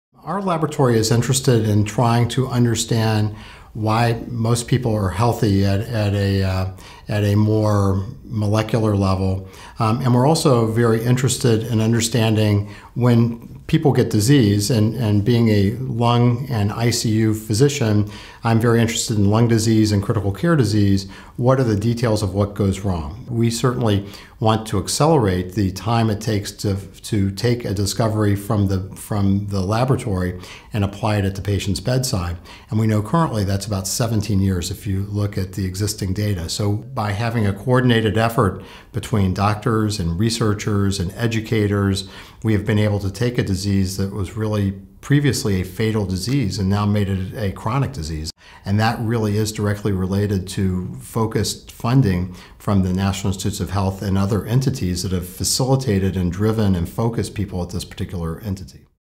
2009 American Recovery and Reinvestment Act: Dr. Clay Marsh
Dr. Clay Marsh, a senior associate vice president for Health Sciences Research at the Ohio State University, discusses the how the 2009 American Recovery and Reinvestment Act has accelerated the time it takes to take a discovery from the laboratory to a patient's bedside.